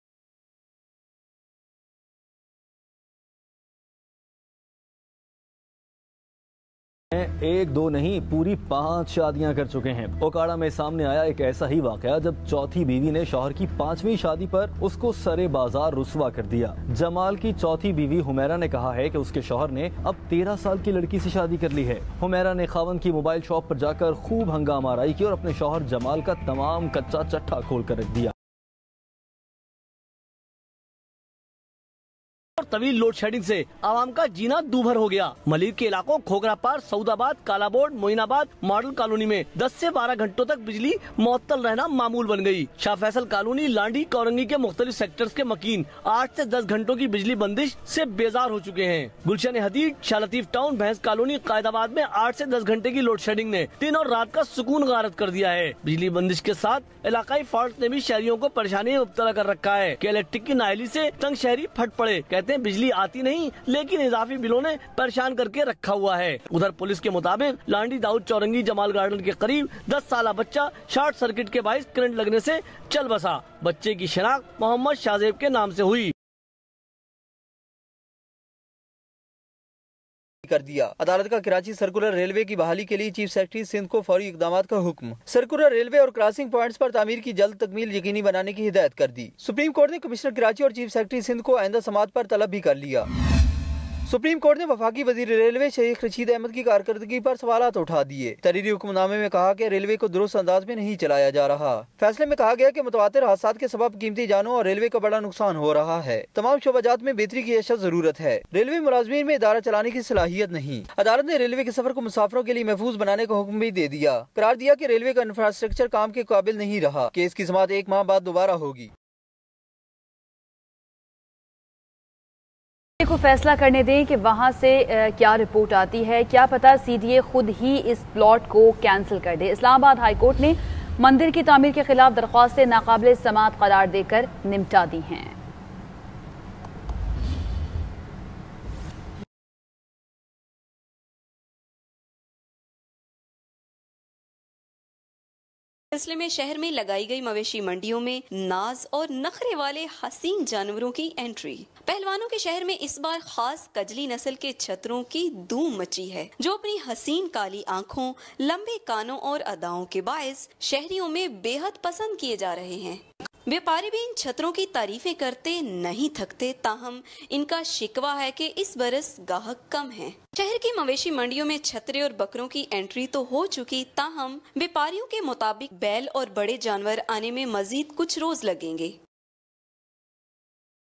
Okara Man Gets Beaten Up By Fourth Wife After His Fifth Marriage
It seems like one marriage is not enough for some people. Islam may allow men to marry four women though how he could take care of them all is a separate debate altogether. In a very bizarre incident, this one man from Okara decided to go beyond and marry for the fifth time.

It didn’t end well for him as his fourth wife publicly beat some sense into him (literally!).

Marriage is a big responsibility. These days, it is already hard enough for people to provide and care for their families. However, some people prefer to take the easy way – start over with a new wife. In a bizarre incident that occurred in the city of Okara, a man was roughed up in public by his previous wife for allegedly marrying a minor.